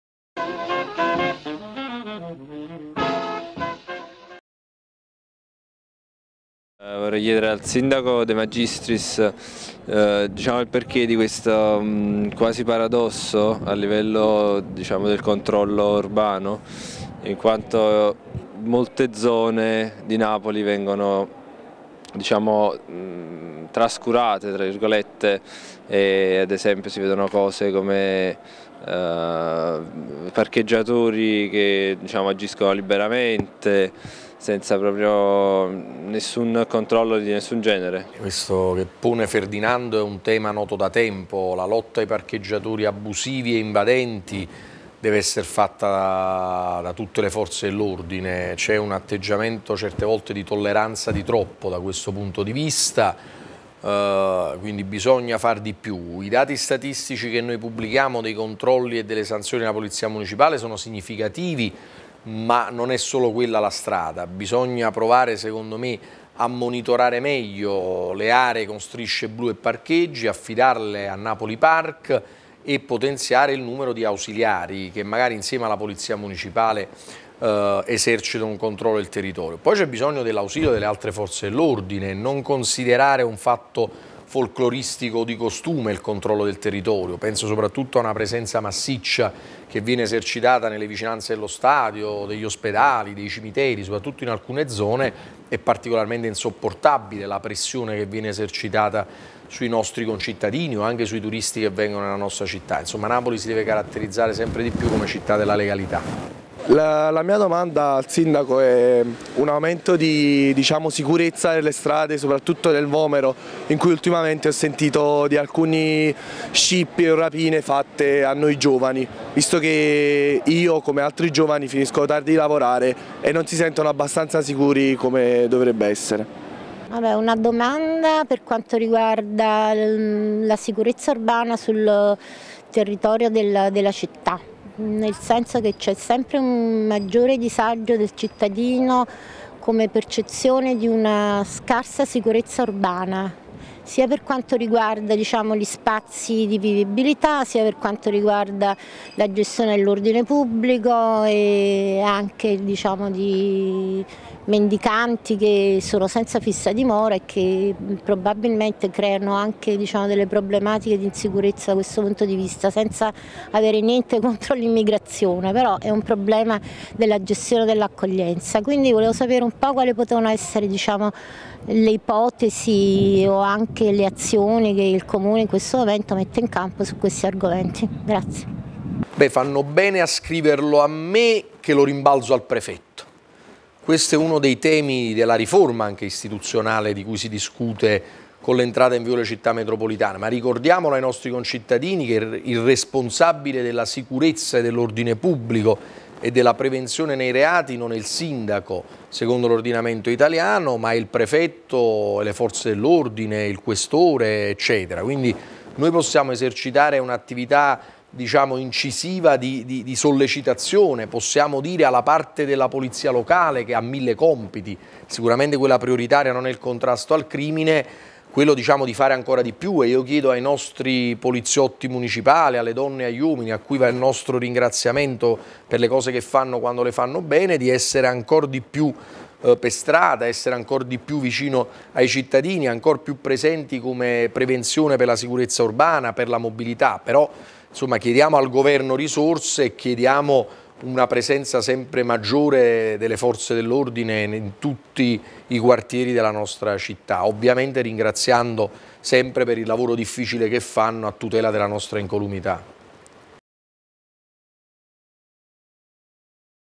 In poche parole - Il sindaco risponde (#11 sicurezza urbana)
Rubrica settimanale a cura dell'Ufficio Stampa del Comune di Napoli. Il sindaco de Magistris risponde ai cittadini sul controllo e la sicurezza urbana.